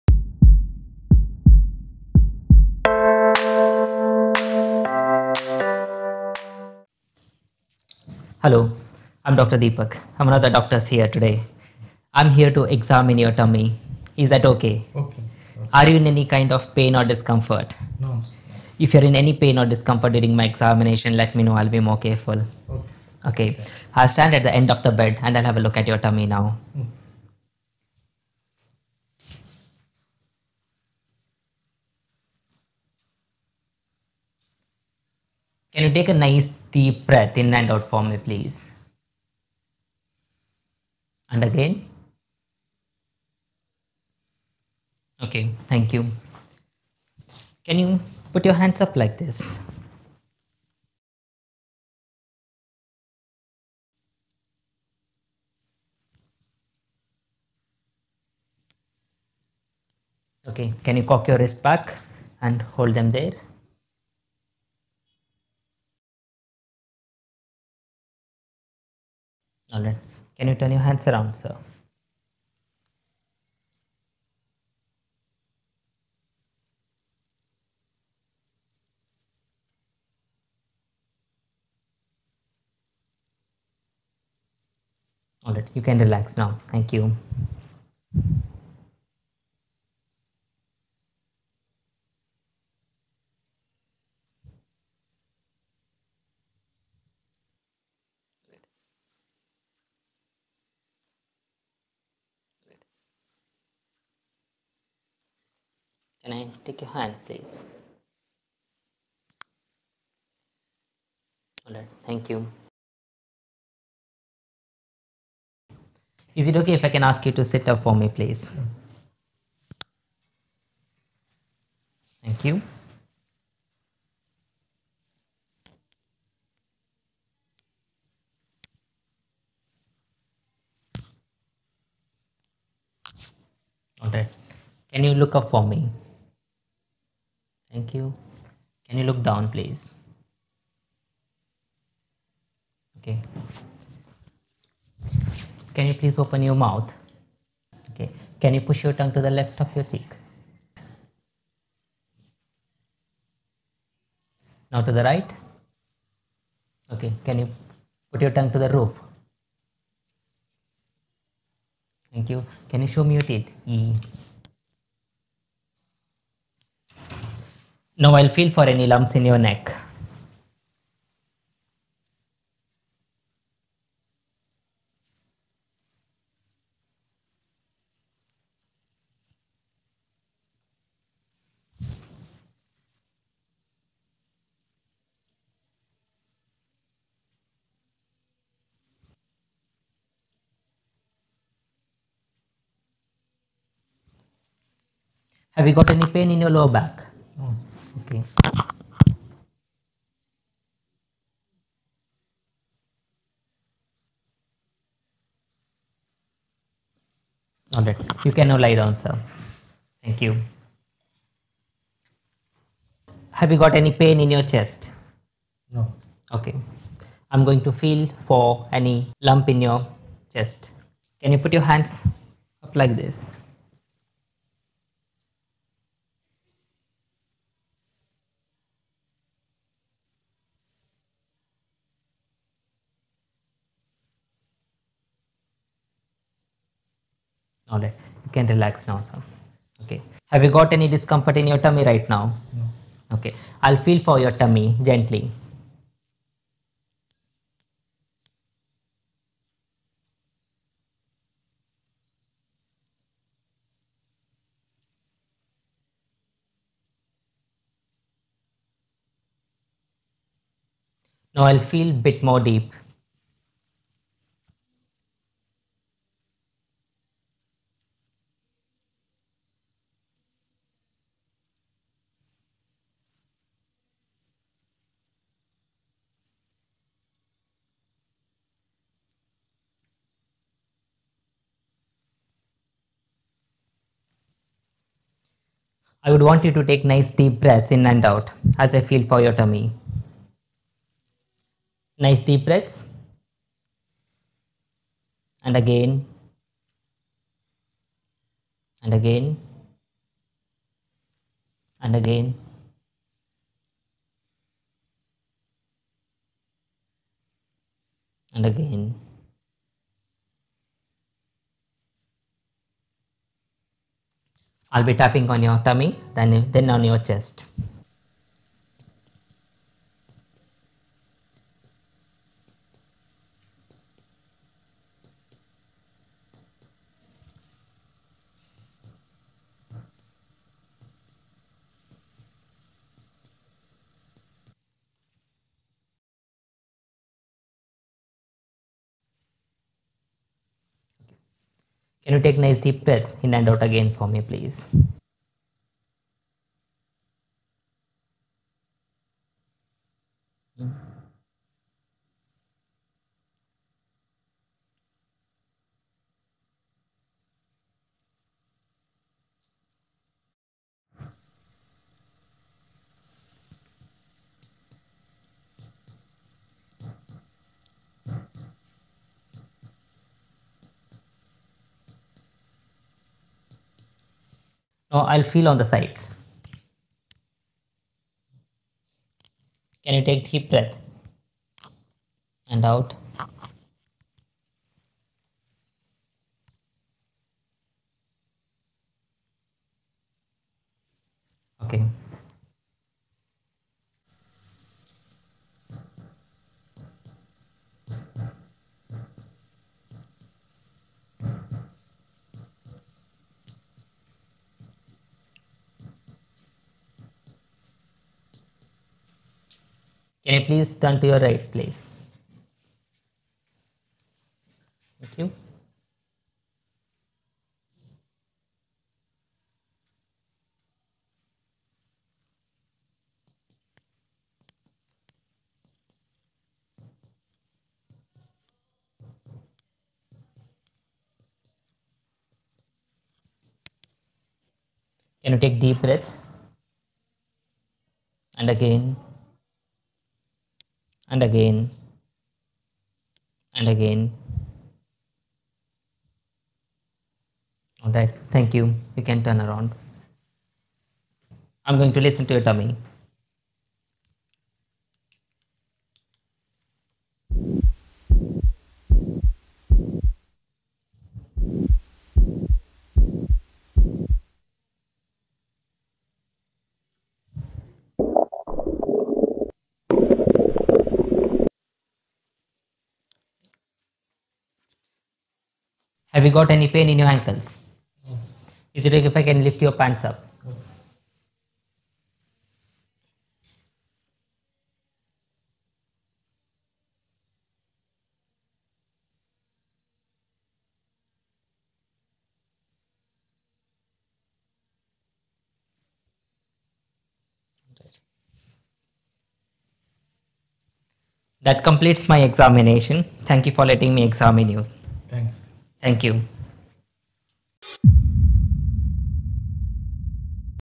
ABDOMINAL EXAMINATION: For PACES / Bedside Medicine.
This video provides a comprehensive guide to the Abdominal Examination: essential for both your PACES exam and everyday clinical practice.

It offers a detailed step-by-step demonstration of the correct technique and sequence, ensuring accurate clinical findings.

Designed to meet high NHS standards, this carefully curated video helps you master the art of clinical examination with precision and confidence.